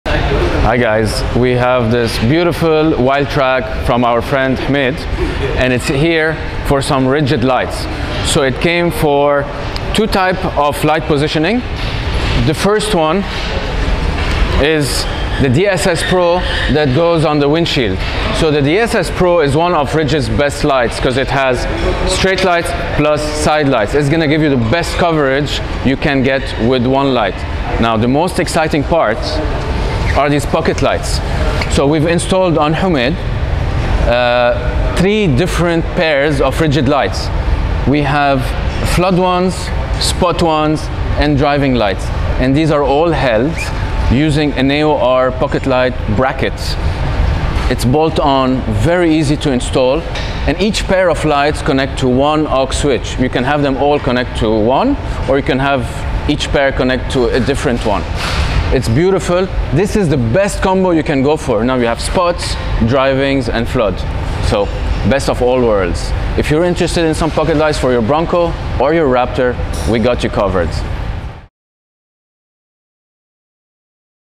Rigid LED Lights (D-SS Pro, Spot, Flood and Driving) on a Ford Bronco - installed at RAMY Automotive
Legendary @RigidIndustriesMedia Industries quality paired with a fabulous @ford Bronco.

We got two types of light positioning:
1. D-SS PRO on the windshield
2. Pocket lights - flood, spot, driving held by @americanoffroad525 light bracket